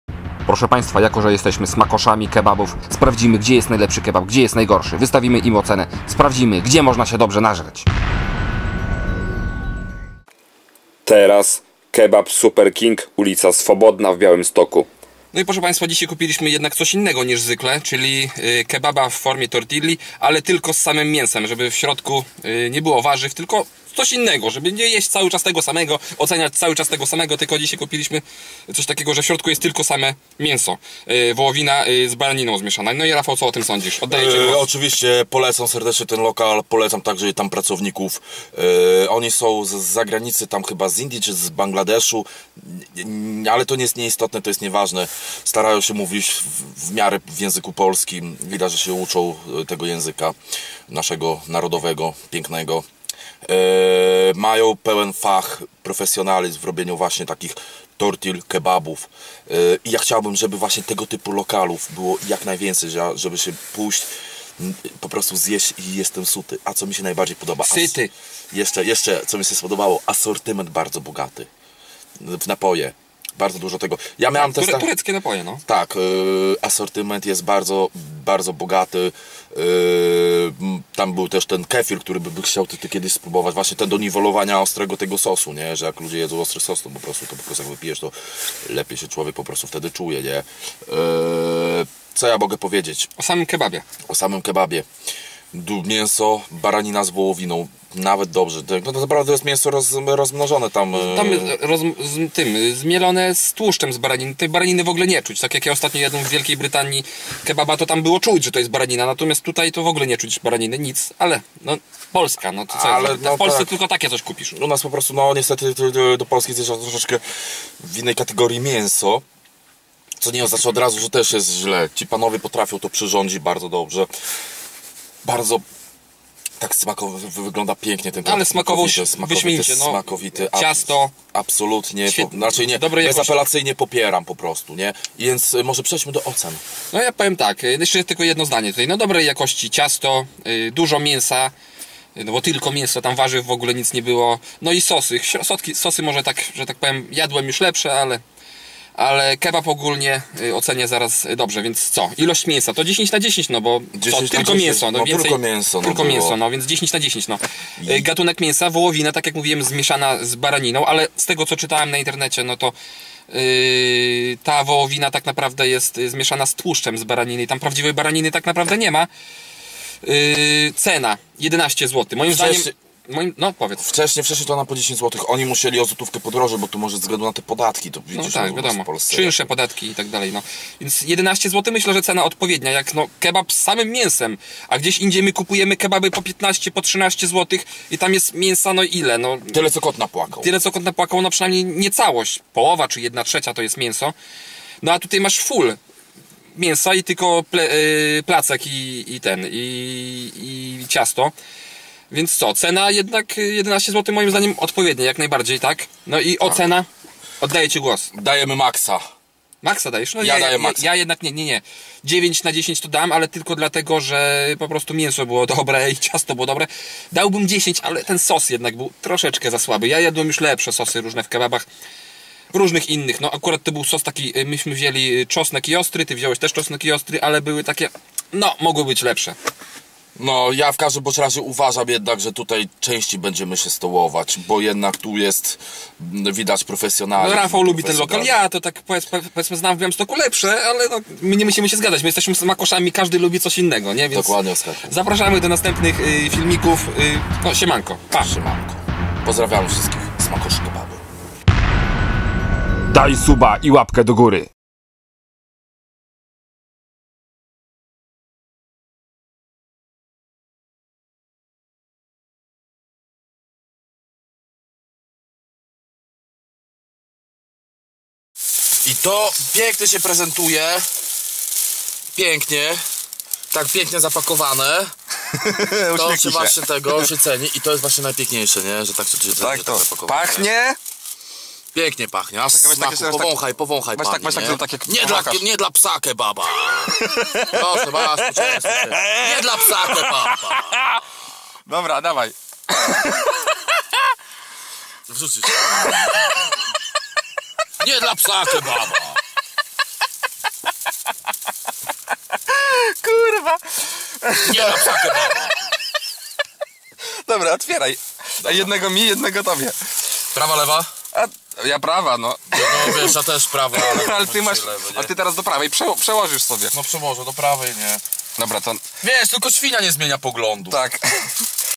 Kebab Super King, ul. Swobodna w Białymstoku, GDZIE DOBRZE SIĘ NAŻREĆ? odcinek 13 - Smakosze kebabów
Dogecoin
D9KeZ1XFgyEUeSY3czLh3qN63sTRKmHVBw

TRON(TRX)
TFJsAj1nfyq4D6GUyFKs4GgvBfKpnqiQSF

Przedstawiają Rafał Dudziński i Oskar Wądołowski

LUTY 2019